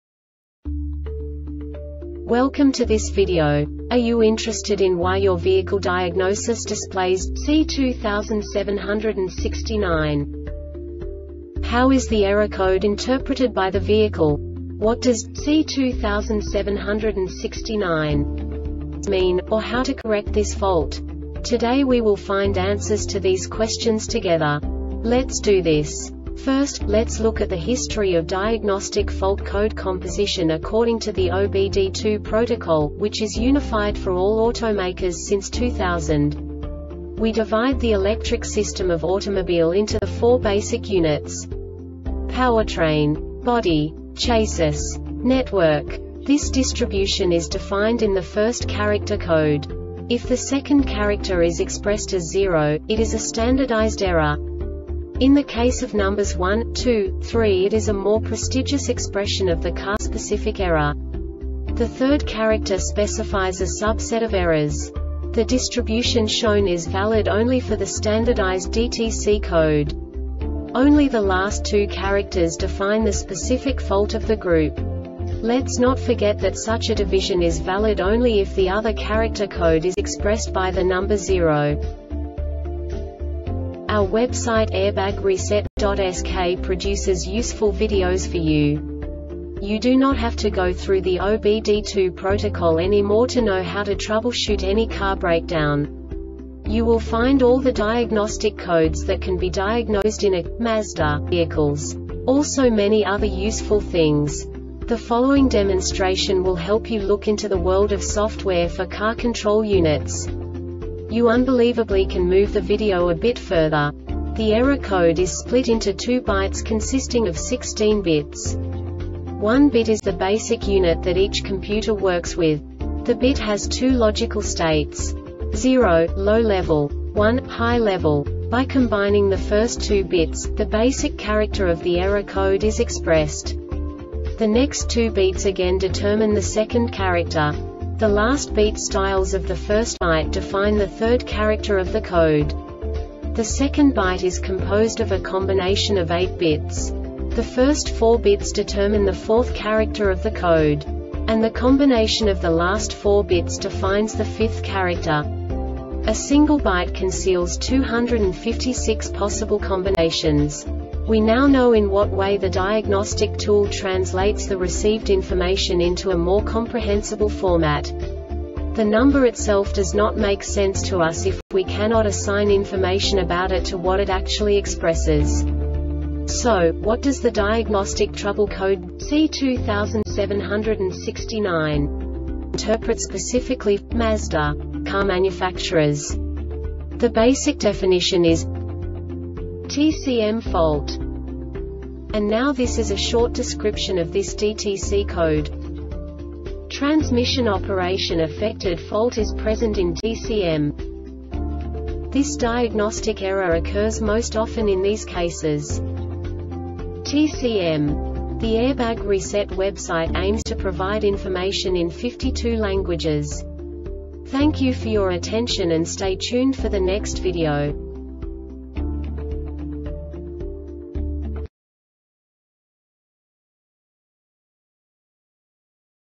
DTC Mazda C2769 Short Explanation
Contents:
0:21 Basic DTC analysis according to OBD2 protocol standard.
1:48 Insight into programming
2:58 A diagnostically understandable description of the Diagnostic Trouble Code
3:05 Basic error definition

Make:
Mazda
Code:
C2769
Definition:
TCM Fault
Description:
Transmission operation affected.Fault is present in TCM.
Cause:
TCM